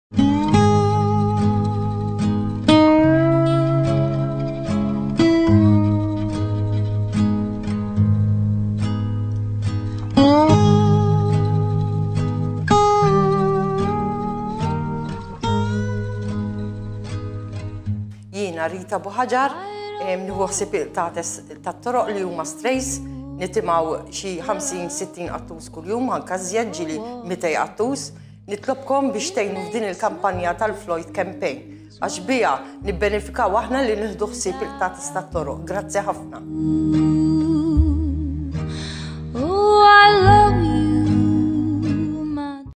The floyd Campaign General Promo Rita Buhagiar
The Floyd Campaign is a philanthropic initiative brought by Pink Panther team Malta , Borg Cardona & Co Ltd and Royal Canin Malta.  The aim of this campaign is part of an on going process by the mentioned organisations to improve animal welfare in general especially the well being of cats and dogs that are currently residing in sanctuaries and shelters and other stray cats and dogs who are found abandoned in the streets.

With your help , Floyd will be doing the following :-
• Provide better nutrition for the cats and dogs in need
• Provide better health and hygene alternatives for the cats and dogs in need
• Provide a better lifestyle for these cats and dogs in need
• Create more awareness towards the animal welfare community.